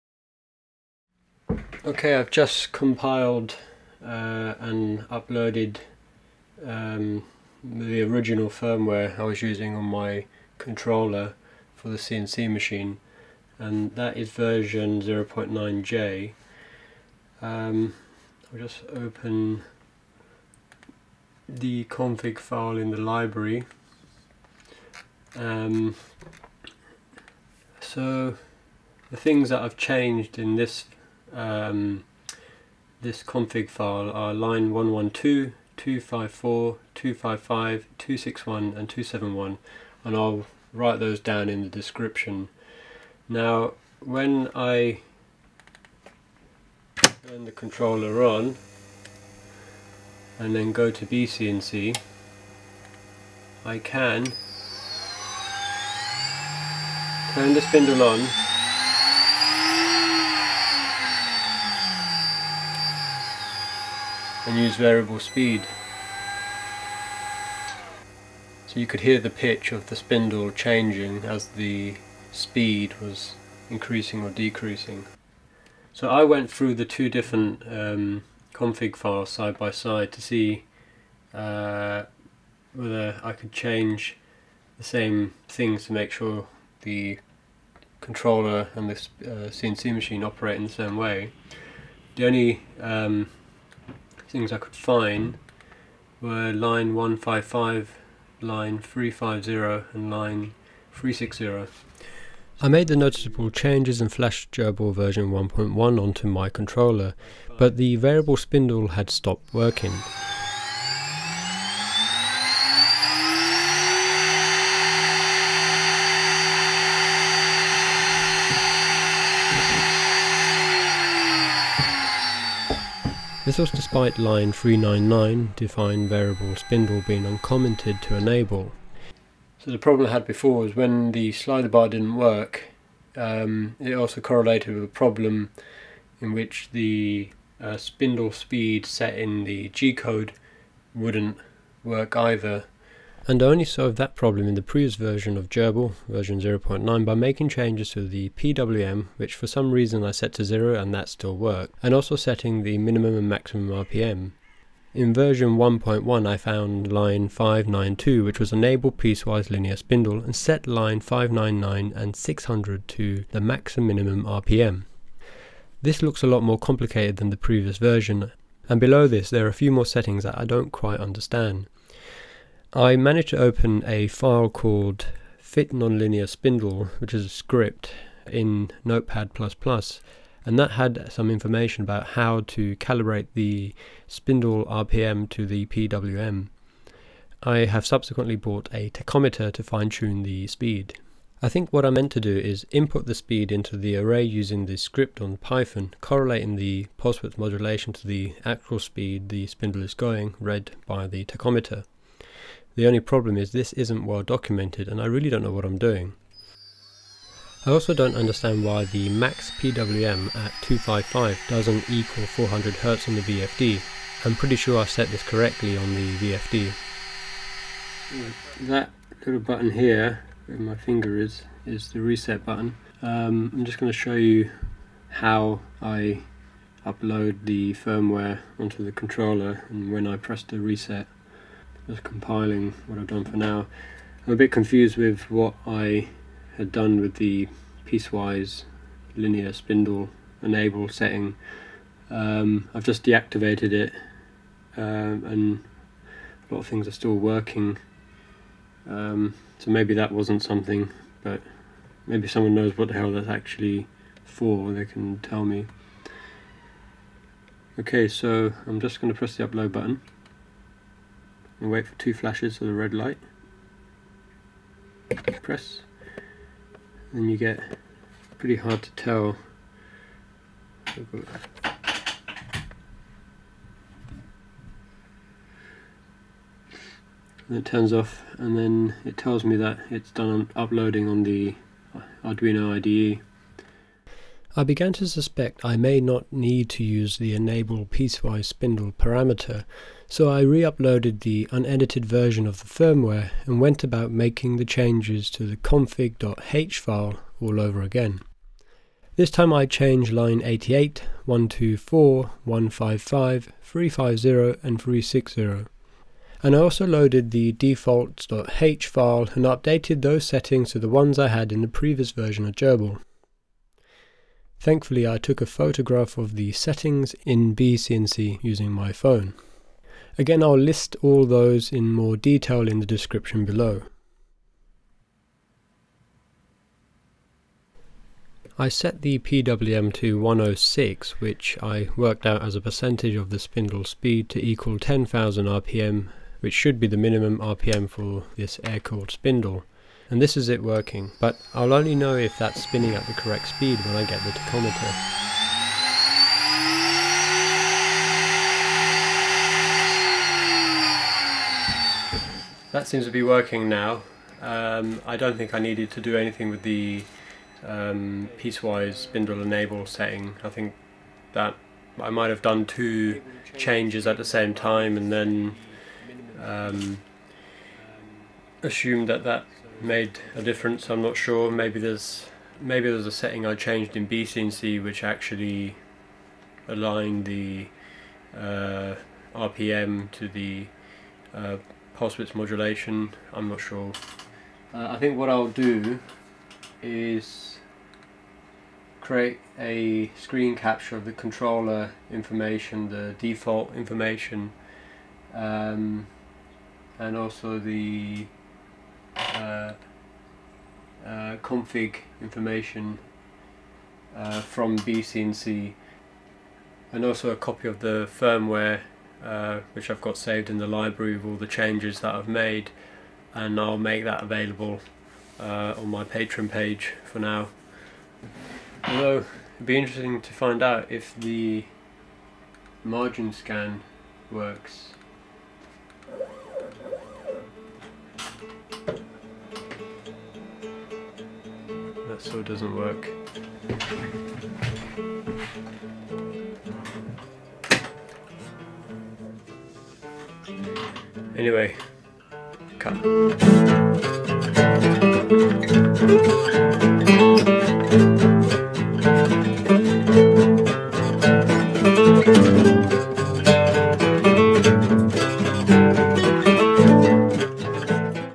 18d Updating Firmware to GRBL V1.1 18b / Phoenix CNC Controller
In this video I update the firmware on my controller from GRBL V0.9 to V1.1 – and attempt to keep settings which will allow the variable speed to continue to perform the way it had. The problem I faced was I couldn’t get the variable speed to work at first in V1.1 – and code looked very different to me. I am a noob at programming and nearly got distracted researching a feature called piecewise_linear_spindle. Anyway I try and show some of the deadends - to start a conversation and hopefully prevent you wasting your time too. I got everything work – apart from the margin scan and here are a list of the codes I changed in V1.1

What I changed in GRBL V1.1

Line Function Status
88: //define HOMING_INIT_LOCK / Comment
124: define HOMING_SINGLE_AXIS_COMMANDS / Uncomment
155: Limits_Two_Swtiches_on_Axes / Uncomment
350: Spindle_PWM_Min_Value 5 / Uncomment and Change to 109
360: Use_Spindle_Dir_as_Enable_Pin / Uncomment